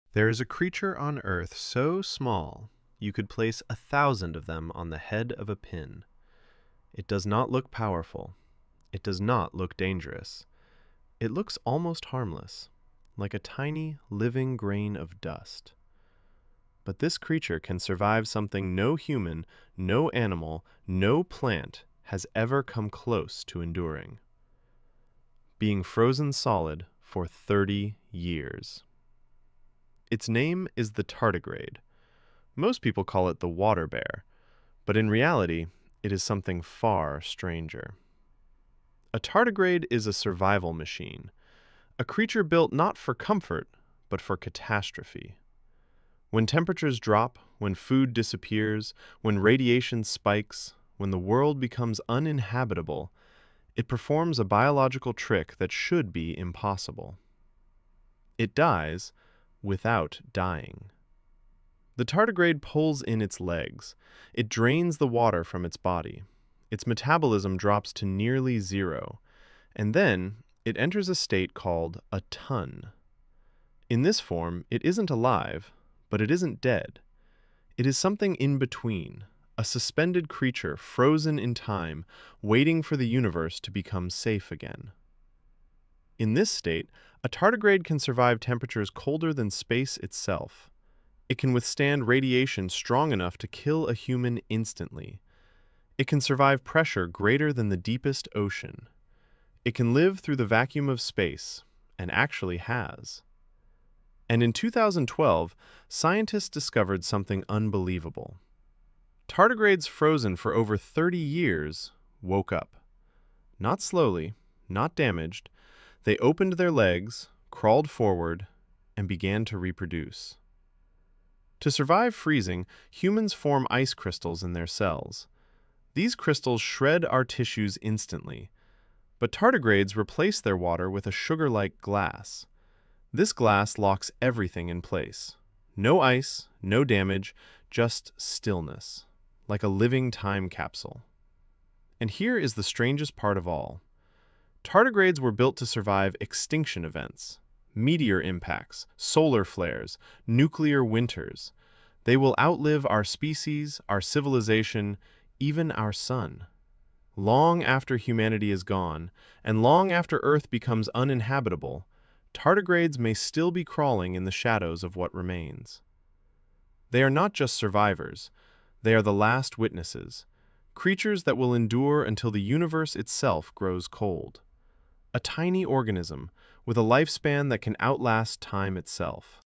The Creature That Can Survive Being Frozen Solid for 30 Years
A creature smaller than a grain of dust… that can shut itself down, freeze into a glass-like shell, and wake up decades later as if nothing happened.
Tardigrades aren’t just survivors— they bend the rules of life itself.
From radiation to the vacuum of space, this microscopic organism endures what should be impossible.

Here’s how a creature that looks almost harmless can outlive extinction events.